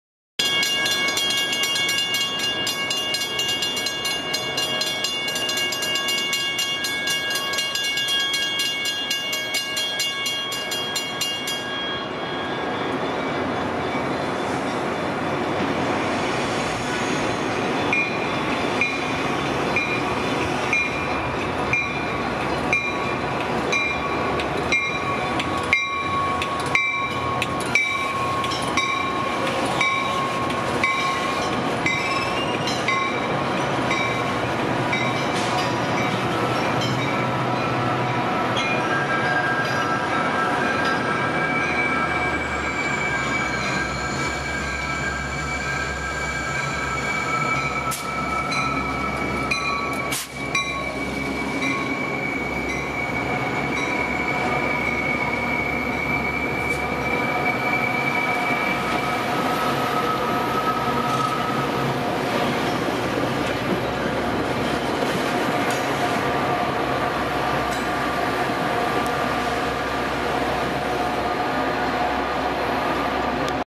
Metra metropolitan area commuter railroad MD Line trains
Metra commuter rail Milwaukee District line diesel-electric locomotive and bilevel coache train runs at Western Avenue to Chicago Union Station in the Illinois, United States.
シカゴ近郊の通勤鉄道メトラのプッシュプルトレインが踏切を通過する。
美國芝加哥通勤鐵路密爾沃基線列車將通過平交道。